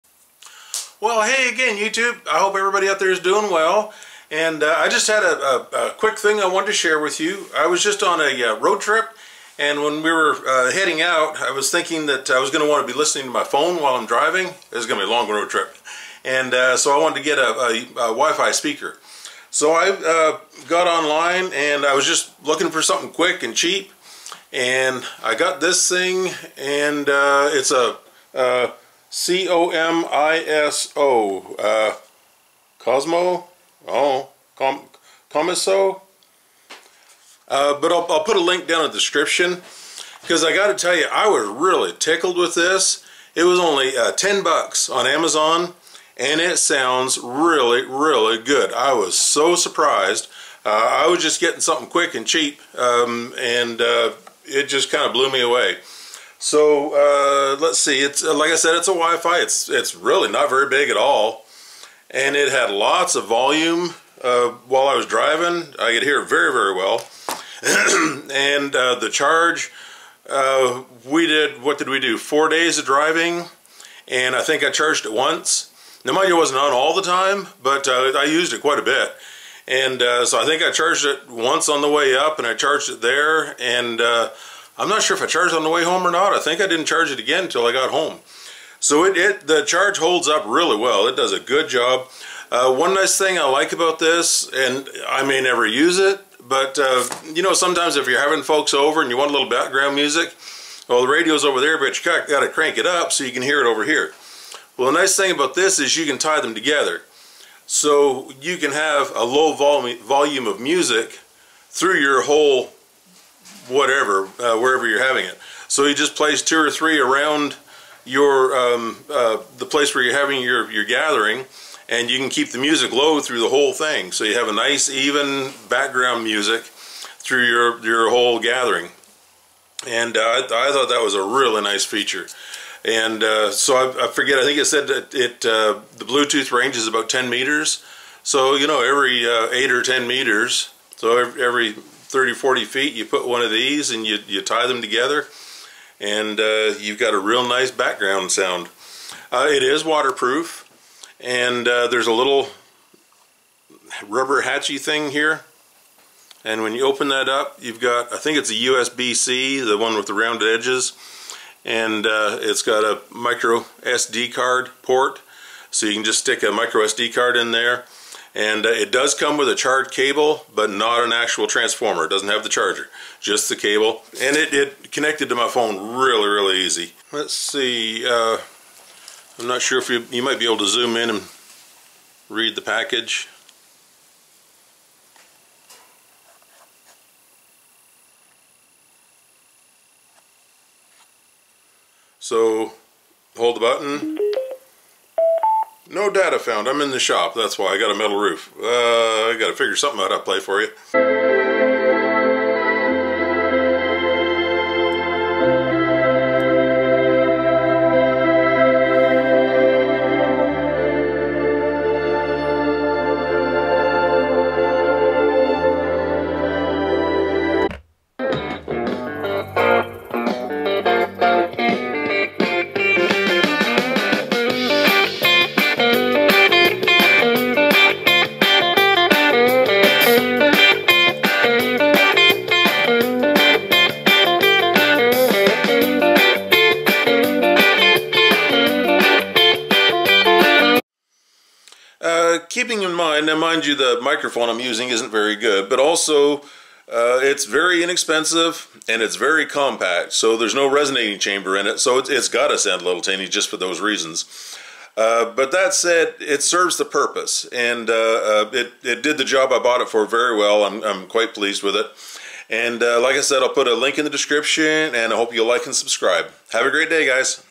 Comiso Bluetooth Speaker from Amazon
If you’re looking for a reliable and high-quality Bluetooth speaker, this review will share my experience with the Comiso X26M.

Link:
comiso Bluetooth Speaker